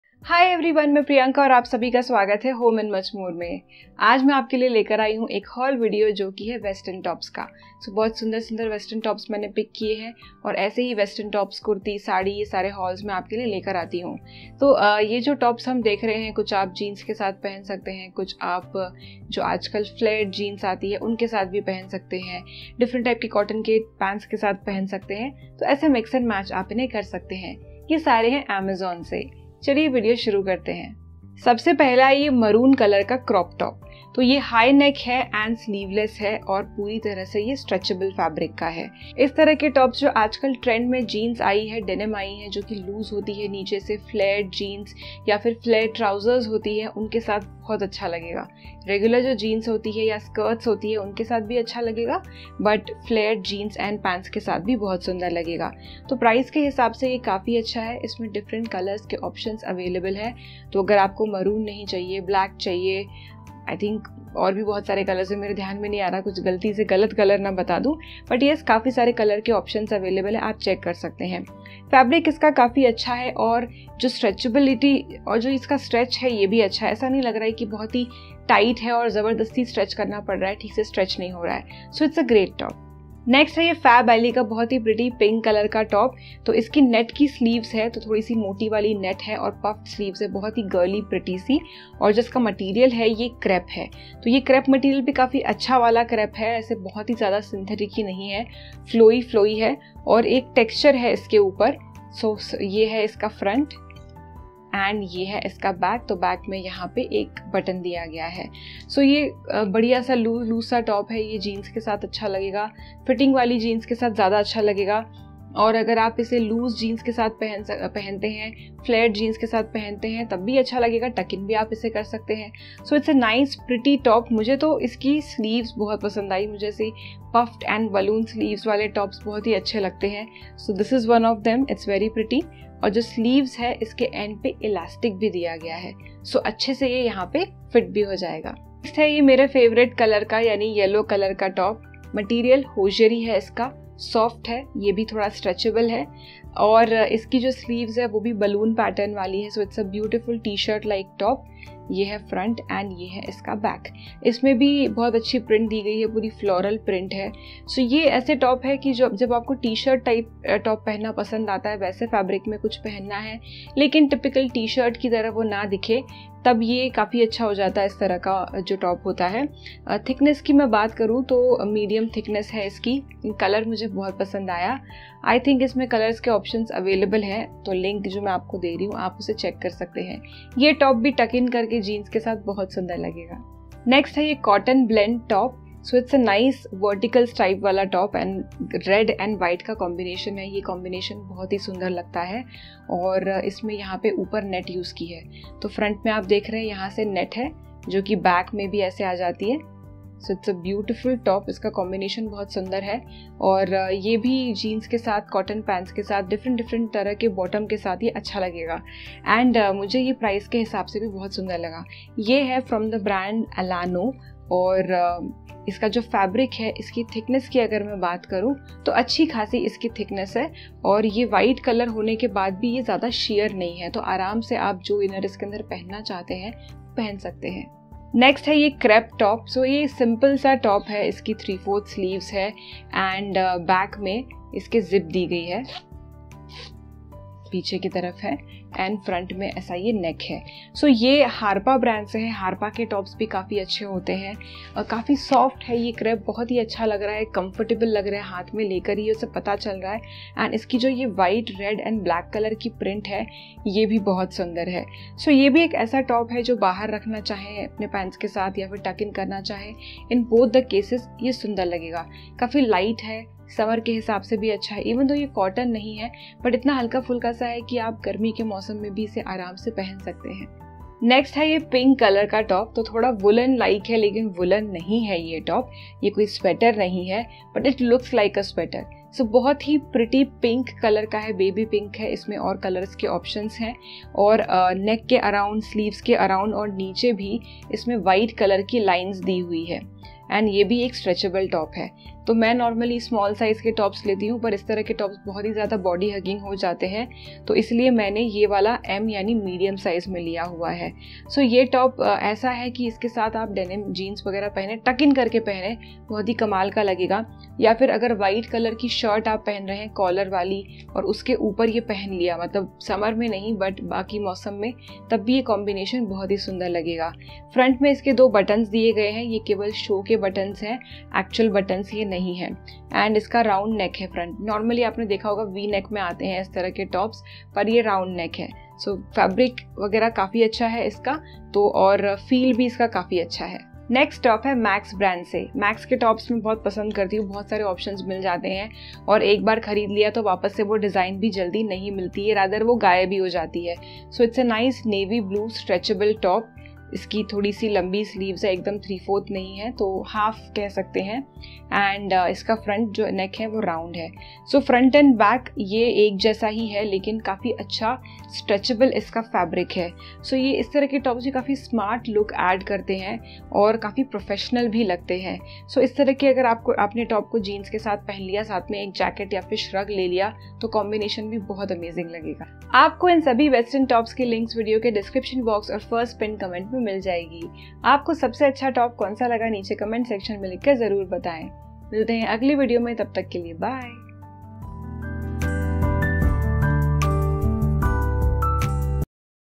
Trendy & Stylish Western Tops | Daily/Office/College Wear Tops Haul from Amazon
Buying Links :

1.  Maroon Brown Sleeveless Top:

2.  Baby Pink Top:

3.  White Top:

4.  Yellow Top:

5.  Pink Organza Sleeves Top:

6.  Navy Blue Top:

7.  Jeans: